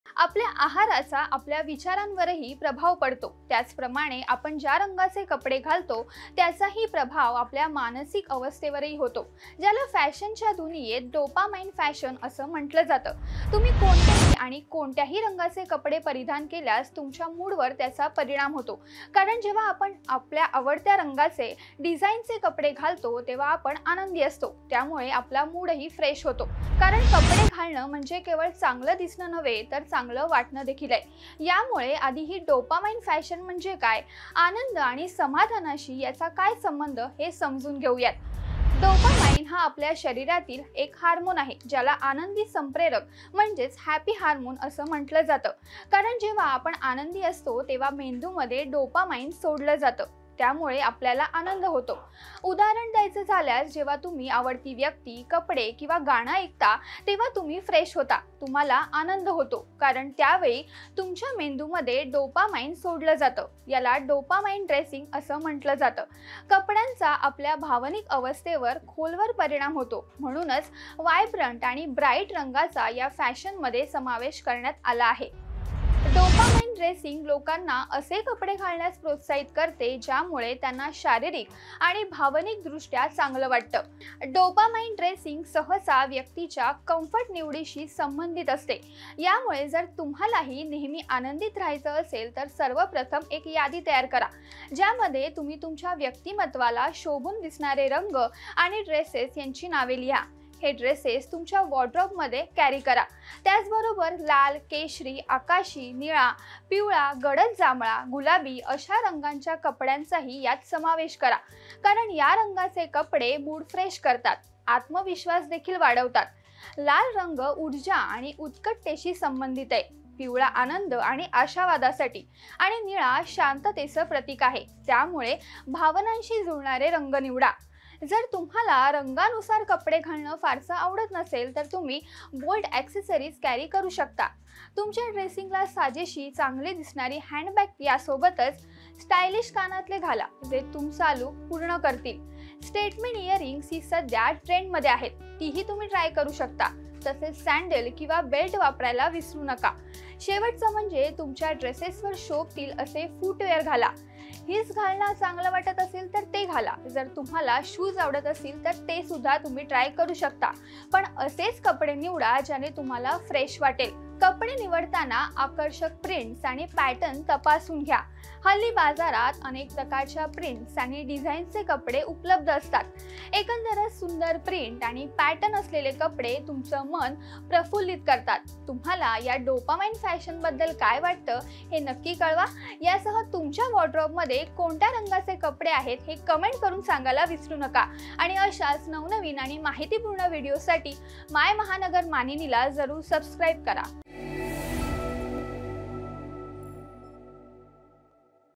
Dopamine Fashion : फॅशनच्या दुनियेत आलेली डोपामाइन फॅशन आहे तरी काय?
तुम्ही कोणतेही आणि कोणत्याही रंगाचे कपडे परिधान केल्यास तुमच्या मूडवर परिणाम होतो. कारण जेव्हा आपण आपल्या आवडत्या रंगाचे, डिझाईनचे कपडे घालतो तेव्हा आपण आनंदी असतो. त्यामुळे आपला मूडही फ्रेश असतो. कारण कपडे घालणे म्हणजे केवळ चांगले दिसणे नव्हे तर चांगले वाटणे देखील आहे. यामुळे आधी ही डोपामाइन फॅशन म्हणजे काय? आनंद आणि समाधानाशी त्याचा काय संबंध आहे. हे समजून घेऊ या.